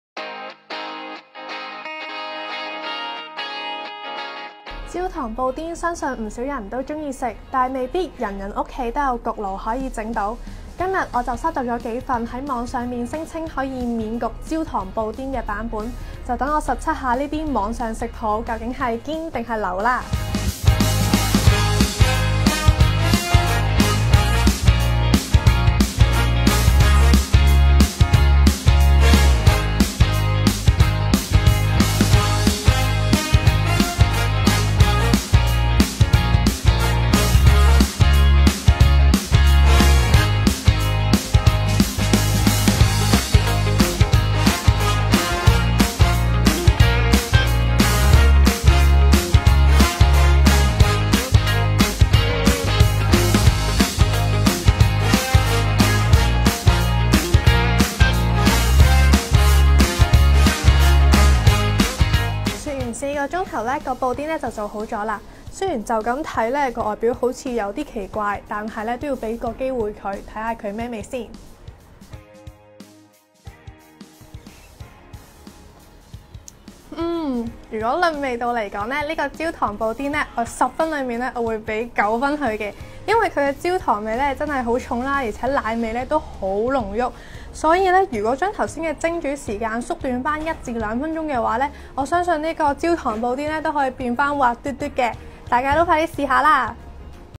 【UFood食譜】實測網上食譜真與假？！免焗焦糖布丁食譜
究竟呢個免焗焦糖布丁又會唔會成功呢？
→ 實測微波爐蛋糕食譜